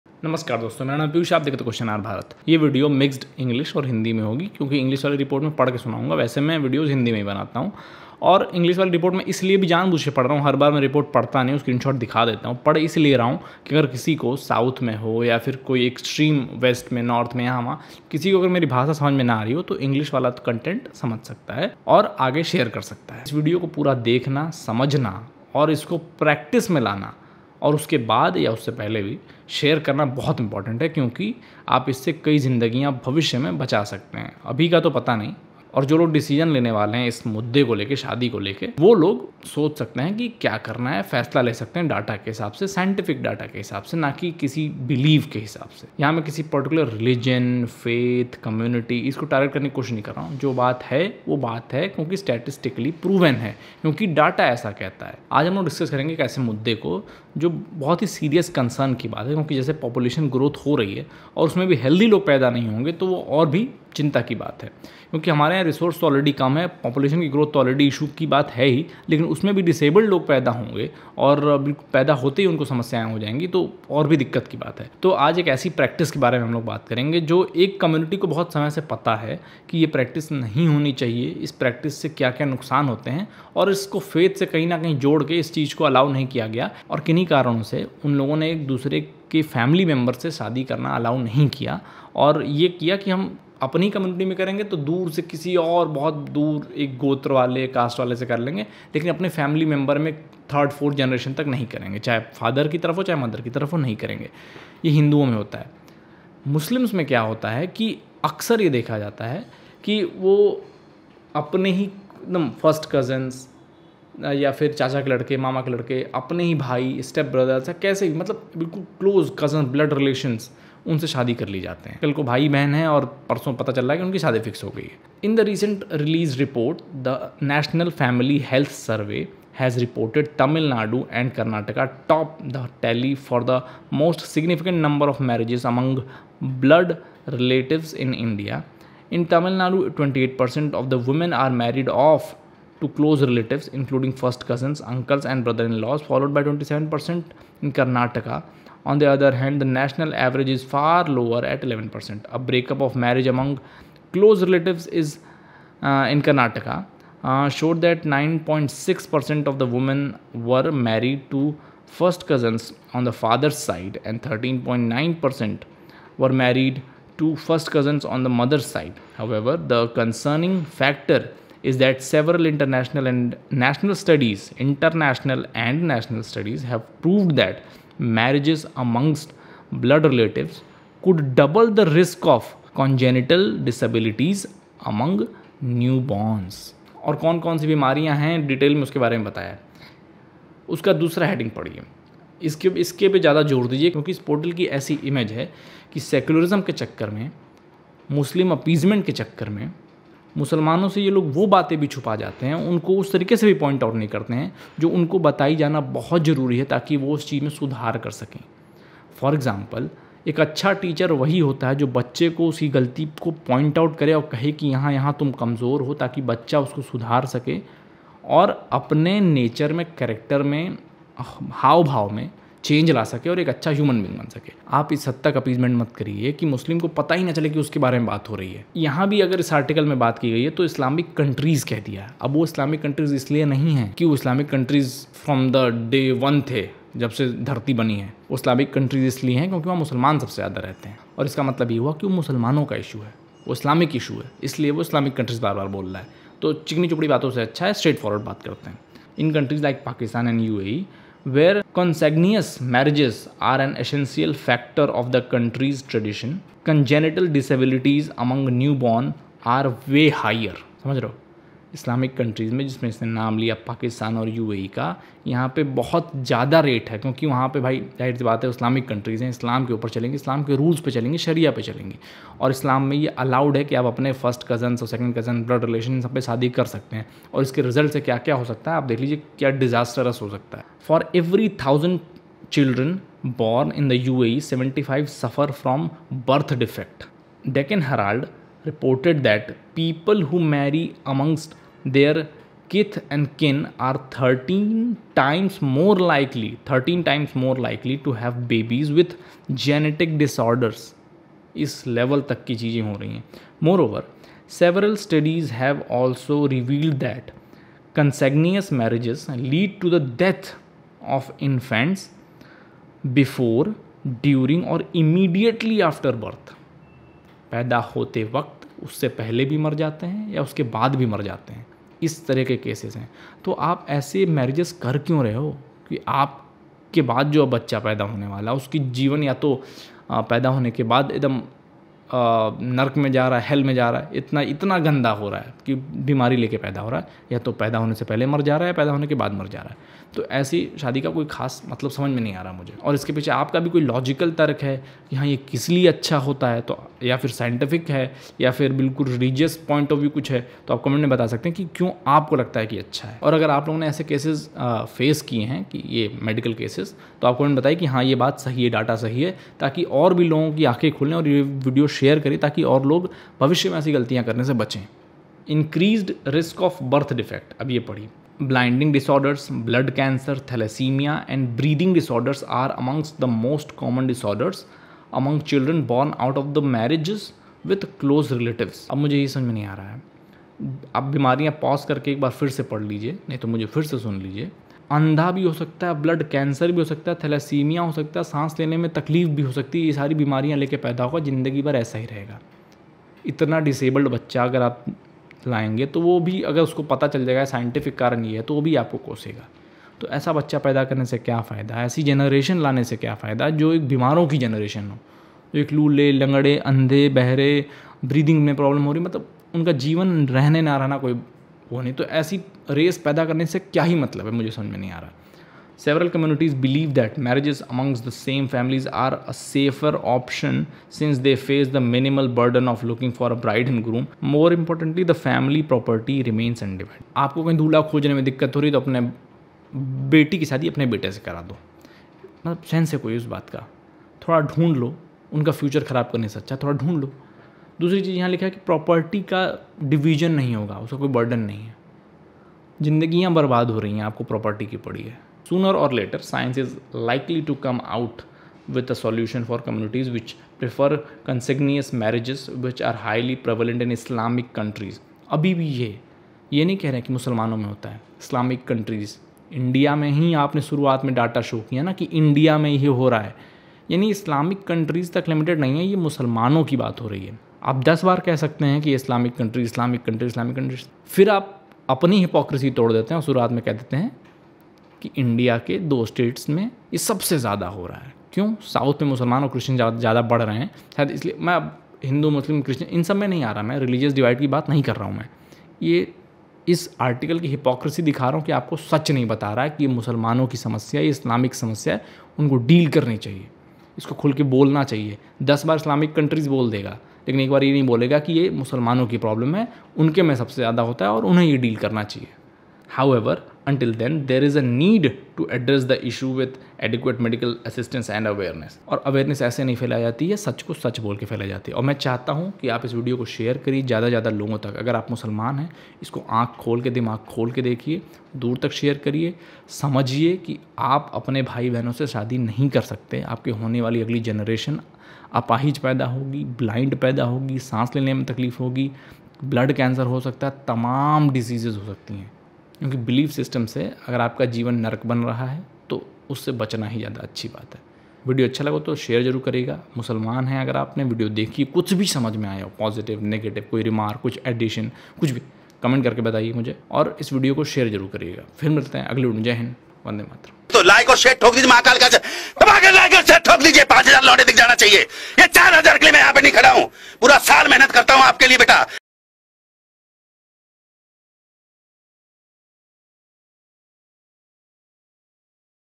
marriage in the same bloodline allowed in islamic law but harmful for humanity | Piyush Rai
marriage in same bloodline as per islamic law ?

Is marriage in the same bloodline not good for your health ?

Muslim marriage Islamic Sharia law

03:12  here muslims comes into the picture

Ways to contribute :
UPI : thepiyushrai7@ybL